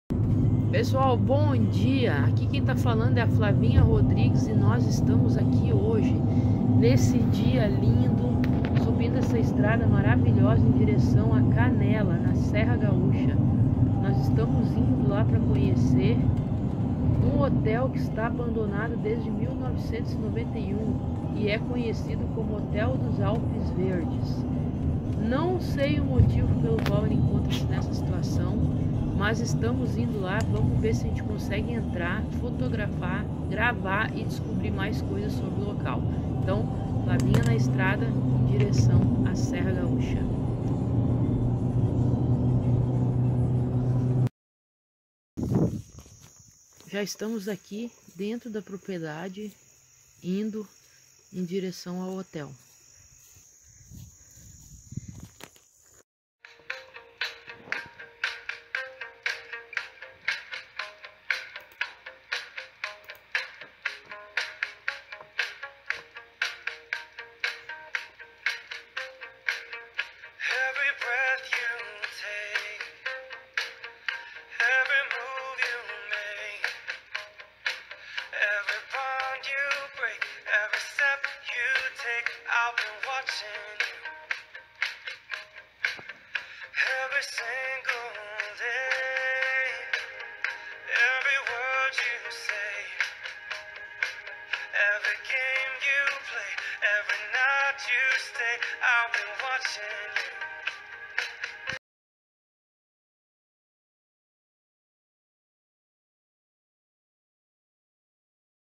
Hotel Abandonado Verdes Alpes
O Hotel esta desativado desde desde 1991. Não descobri o motivo, se alguém souber posta os comentários...